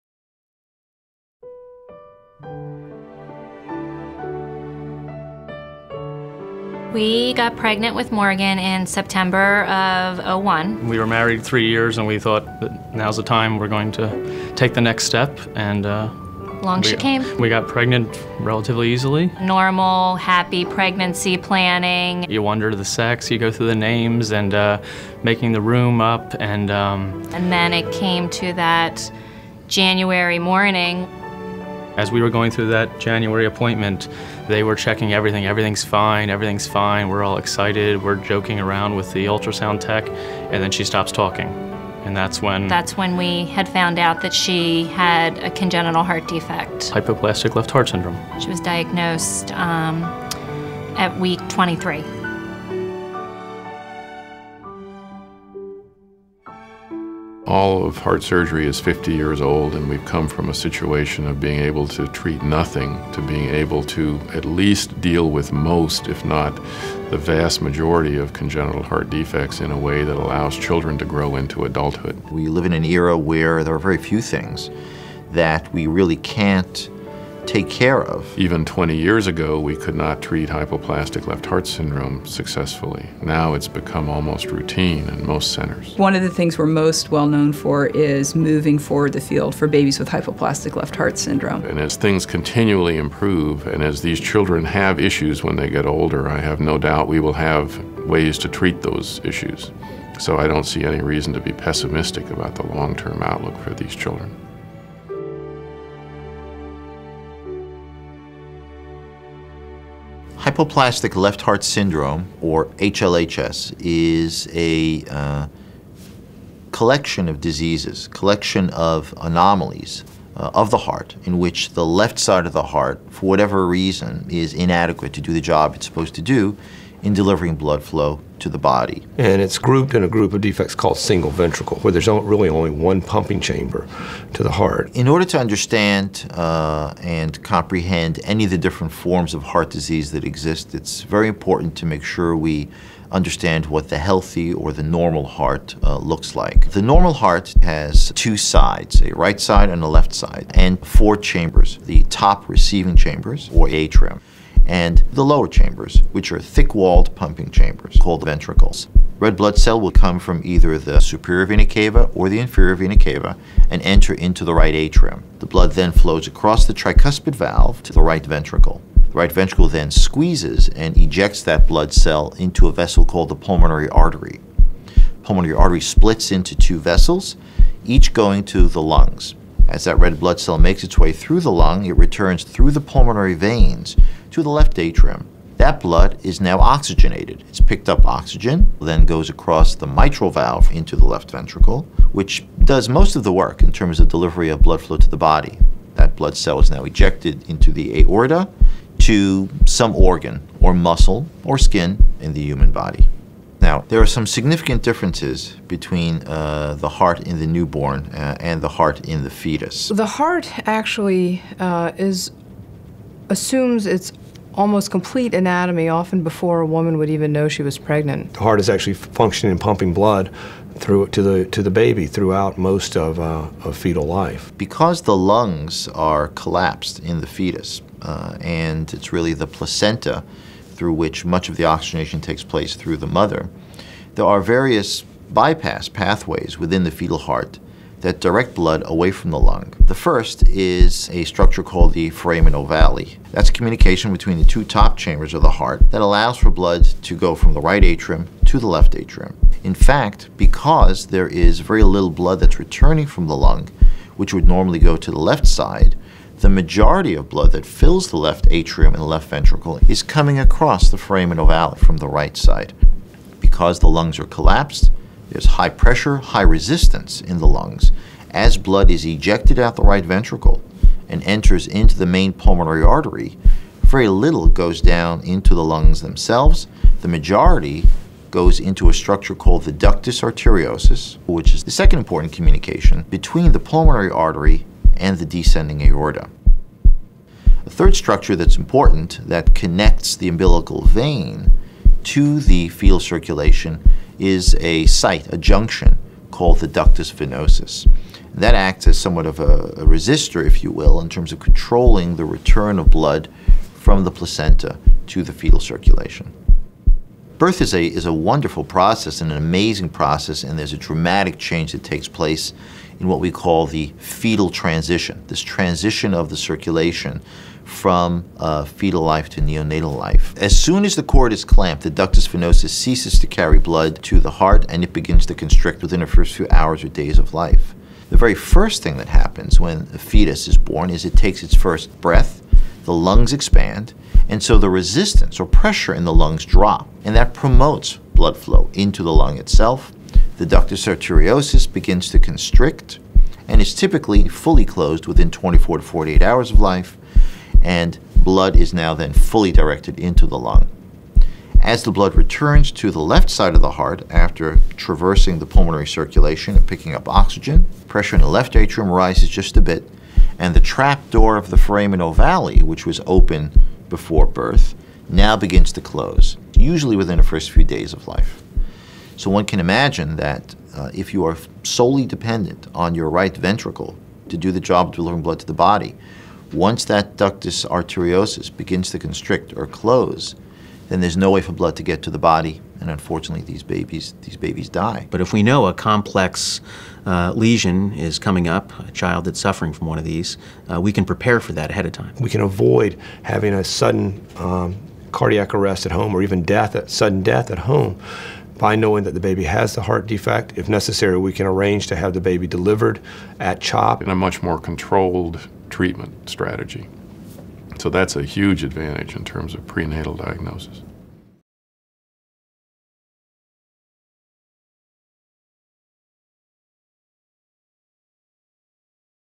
Explaining Hypoplastic Left Heart Syndrome - HLHS (1 of 5)
Andrea and Shane talk about their experiences with daughter Morgan's prenatal diagnosis of hypoplastic left heart syndrome (HLHS), a severe congenital heart defect that involves an underdeveloped left ventricle. Expert pediatric cardiologists from The Children's Hospital of Philadelphia explain how the normal heart develops and functions, and the advantages of prenatal diagnosis in treating babies with a single ventricle defect like HLHS.

Learn more about how HLHS is treated at The Children's Hospital of Philadelphia